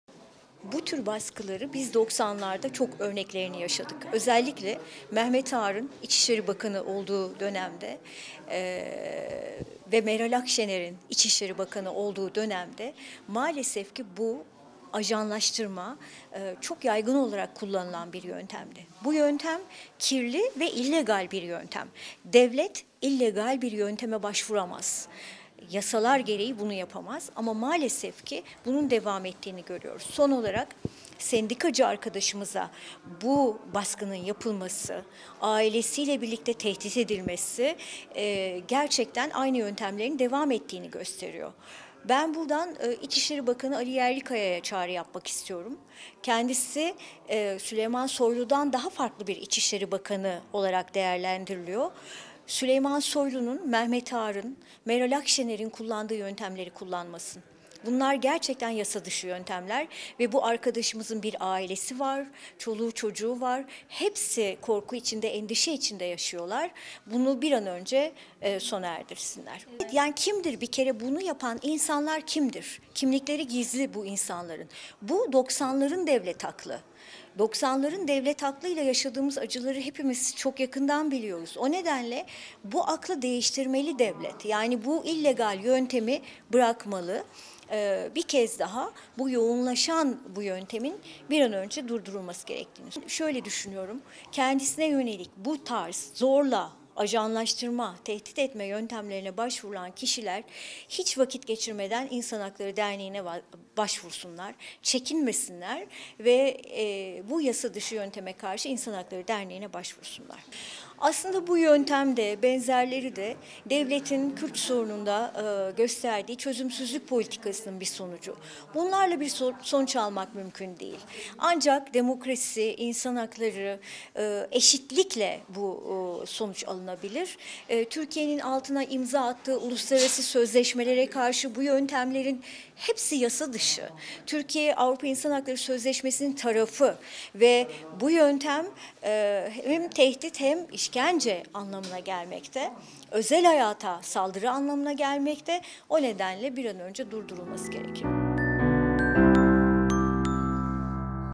EREN KESKİN'DEN İÇİŞLERİ BAKANI'NA: SOYLU, AĞAR VE AKŞENER’İN YÖNTEMLERİNİ KULLANMA
İHD Eş Genel Başkanı Eren Keskin, son yıllarda örnekleri ile sıkça karşılaştıkları ajanlaştırma politikasına dair İçişleri Bakanı Ali Yerlikaya’ya, "Kendisini Süleyman Soylu’dan daha farklı bir İçişleri Bakanı olarak değerlendiriliyor. Soylu’nun, Mehmet Ağar’ın, Meral Akşener’in kullandığı yöntemleri kullanmasın" diyerek seslendi